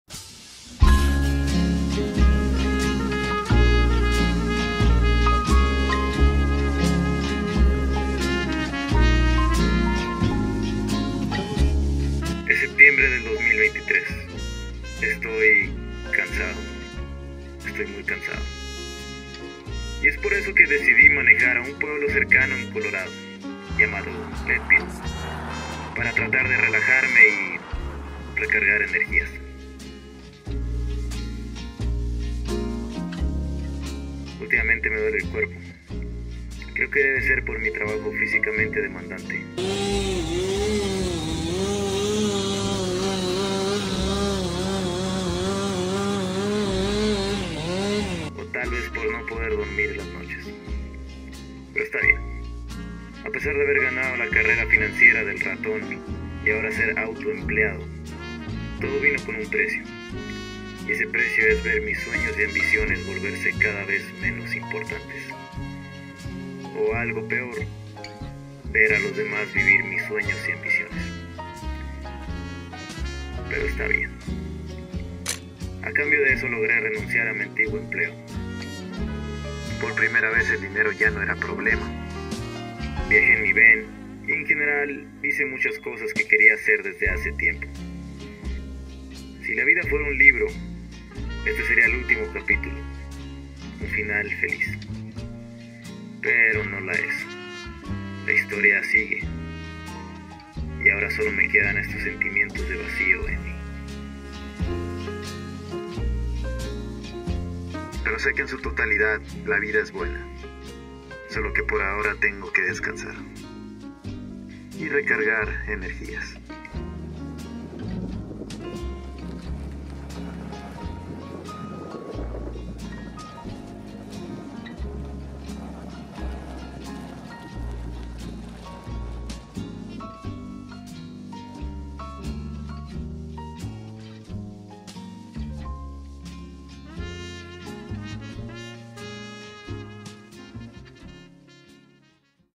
Escápate de Todo…| TRAVEL VLOG
Cuando la vida te abruma, es bueno tener un escape, ya sea con alguien, con algo o algun lugar. Para mi era montarme en mi van y manejar al pueblo mas lejano que encontrara, siempre me sirvio para recargar energias y empezar de nuevo.